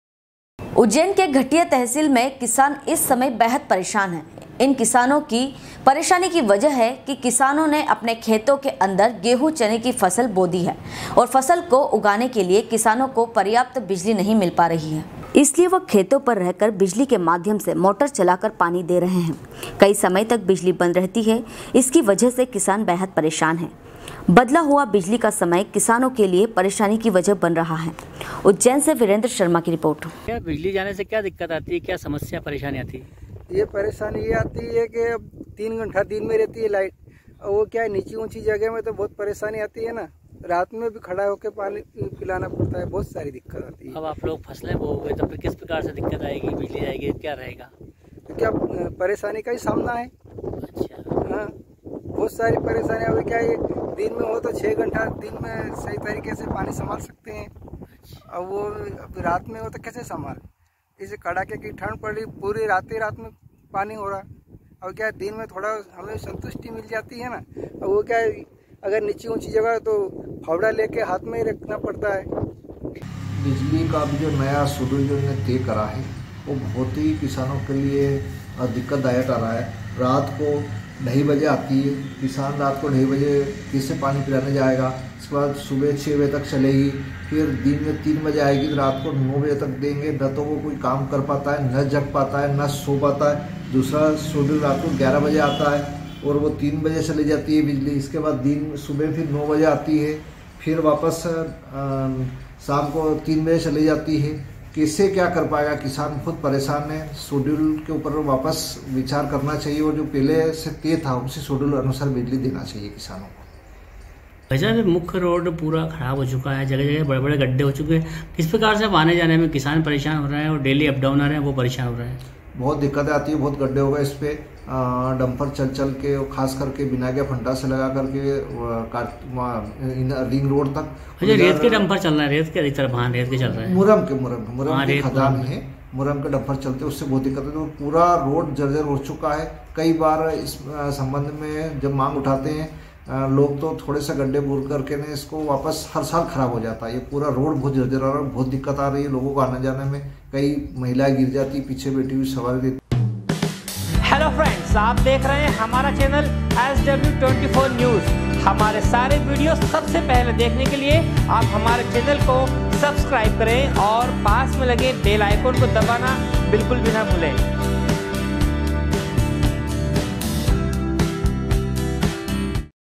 SW 24 News Madhyapradesh
ujjain उज्जैन के घटिया तहसील में किसान इस समय बेहद परेशान हैं इन किसानों की परेशानी की वजह है कि किसानों ने अपने खेतों के अंदर गेहूं चने की फसल बो दी है और फसल को उगाने के लिए किसानों को प्रयाप्त बिजली नही मील रही है